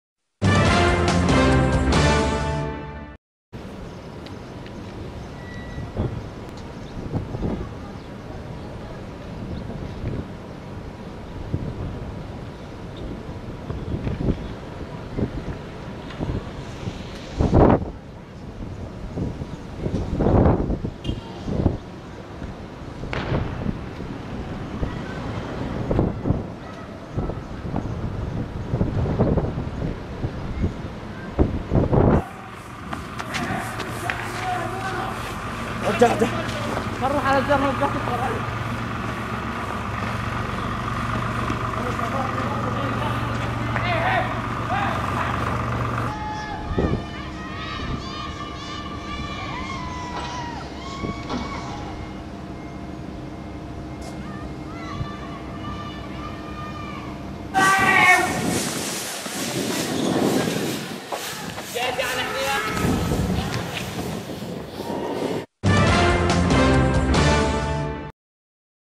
البحرين : قوات النظام تغرق الأحياء السكنية بالغازات السامة - النويدرات 15/6/2013
قوات النظام الخليفي تغرق الأحياء السكنية بالغازات السامة ومسيلات الدموع عصر امس السبت (15 يونيو/حزيران 2013) تزامنا مع فعالية "اللحظة الحاسمة 3" حيث يعتصمون جميع البحرينيين أمام منازلهم للمطالبة بحق الشعب في تقرير مصيره واختيار النظام الذي يحكمه.